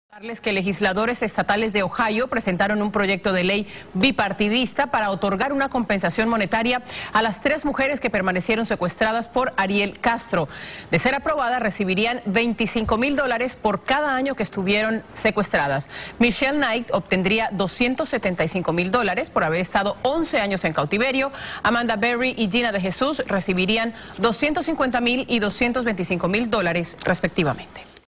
Proponen compensación monetaria para las víctimas de Ariel Castro -- Noticiero Univision
Legisladores de Ohio propusieron otorgan una compensación a las 'Cautivas de Cleveland', podrían recibir hasta 275 mil dólares.